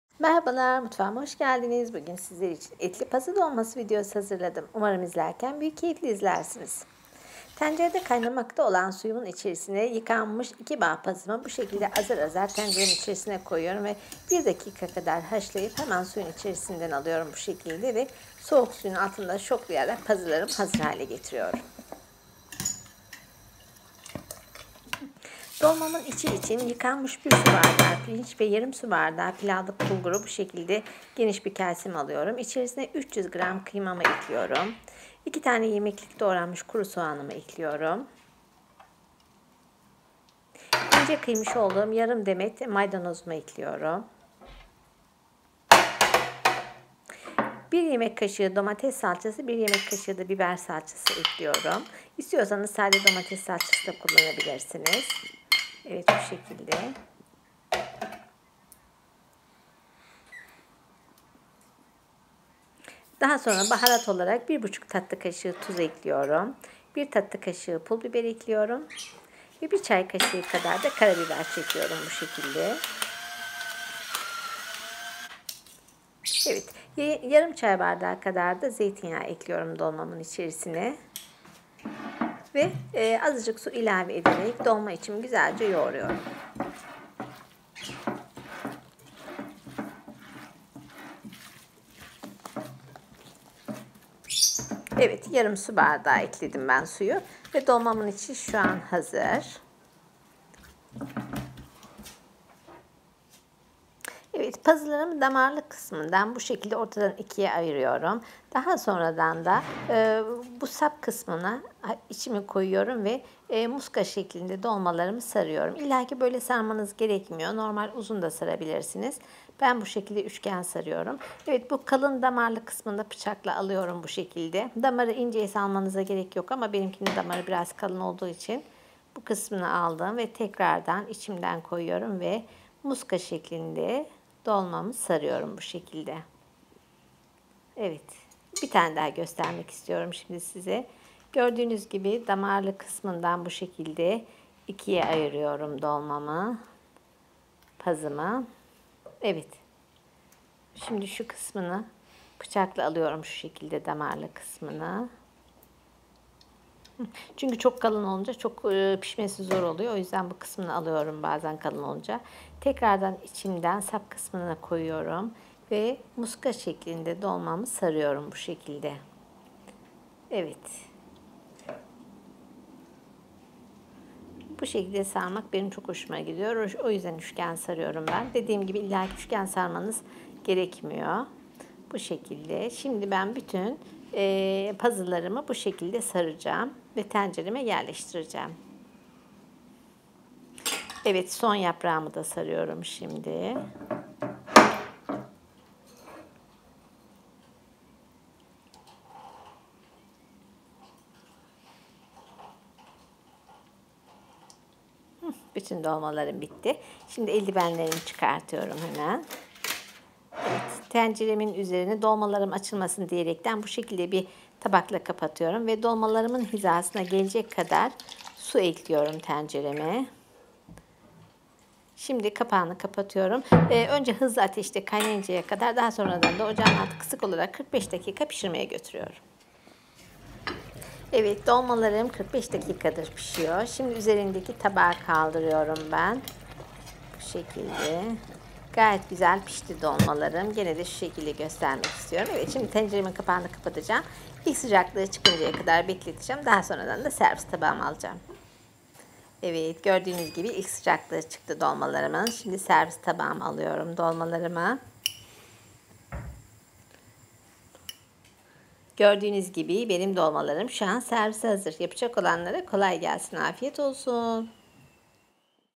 ETLİ PAZI DOLMASI
Malzemeler:
2 bağ pazı
300 gr kıyma
1 su bardağı pirinç
Yarım su bardağı pilavlık bulgur
2 tane kuru soğan
Yarım demet maydanoz
1 yemek kaşığı domates salçası
1 yemek kaşığı biber salçası
1,5 tatlı kaşığı tuz
1 tatlı kaşığı pul biber
1 çay kaşığı kara biber

Yapılışı: Tencereye 1 litre su konulup kaynatılır. Yıkanmış pazılar azar azar kaynamış suya konulup 1 dakika haşlanıp kenara alınır. Bütün pazılar haşlanınca soğuk suya batırılıp süzgece alınır.
Dolamanın için hazırlamak için yıkanmış pirinç ve bulgur geniş bir kaba alınır. Üzerine yemeklik küçük doğranmış soğanlar, ince kıyılmış maydanoz, salçalar, baharatlar, zeytinyağı ve yarım su bardağı su ilavesi ile iç malzemesi güzelce yoğrulur. Pazılar damar kısımlarından ikiye ayrılır ve sap kısmına iç malzemesinden konulup üçgen şeklinde sarılıp tencereye dizilir. Pazının diğer yarısındaki damarlı kısım bıçak ile kesilir ve sap kısmına iç konulup sarılır. Bütün iç malzemesi ve pazılar sarılıp tencereye yerleştirilir. Daha sonra tenceredeki dolmaların üzeri tatlı tabağı ile kapatılır ve dolmaların üzerini geçecek kadar su konulup tencerenin kapağı kapatılarak ocağa alınır. Dolmalar hızlı ateşte kaynayınca ocağın altı kısılır ve dolmalar 45 dakika pişirilir. Bu arada suyu kontrol edilir, suyunu çekmiş ise su ilave edilir. Dolmaların ilk sıcaklığı çıkınca servis yapılır. Afiyet olsun.